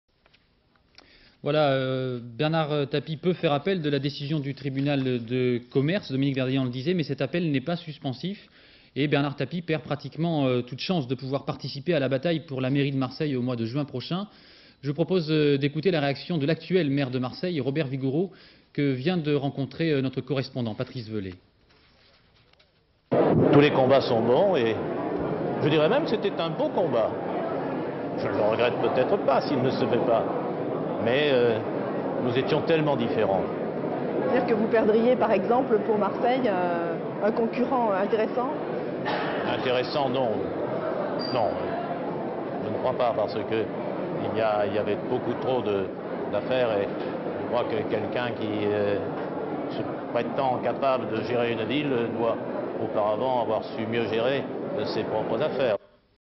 Réaction Vigouroux
14 decembre 1994
Après l'annonce de mise en liquidation personnelle de BERNARD TAPIE et de son épouse, réaction sur le vif de ROBERT VIGOUROUX maire de MARSEILLE : "Tous les combats sont bons, c'était un bon combat... Nous étions tellement différents." Q : "Est-ce la perte d'un candidat intéressant ?" R : "Intéressant ? Non, beaucoup trop d'affaires." Images d'archive INA